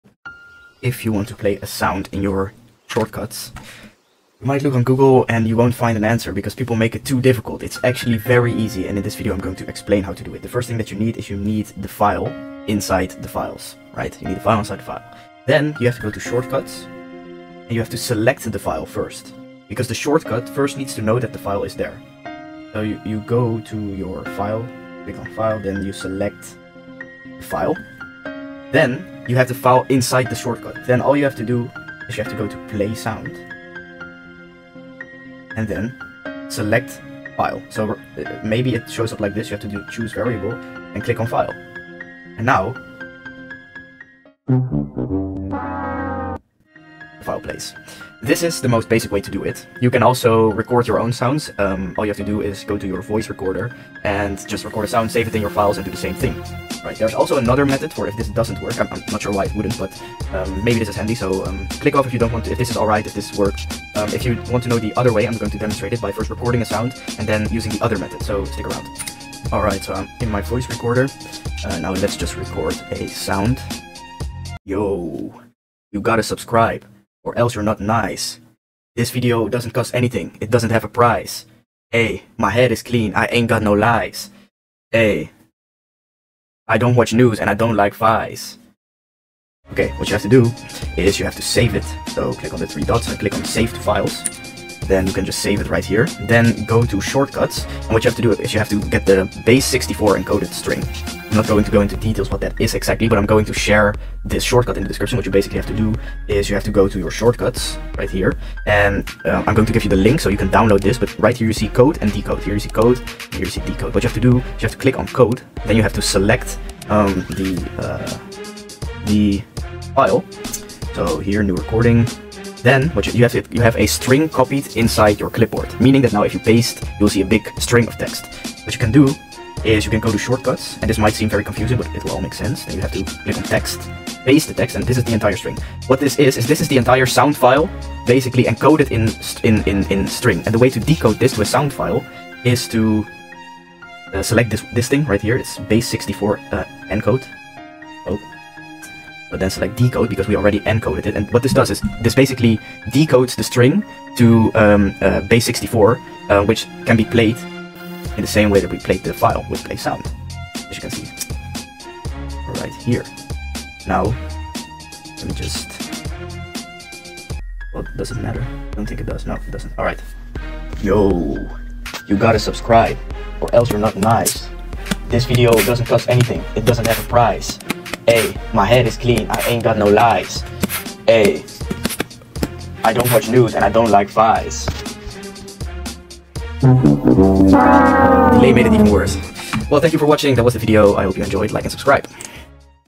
How to play whatever sound in iPhone Shortcuts 1 Minute Tutorial (all you need to know)
Shortcuts Code Base64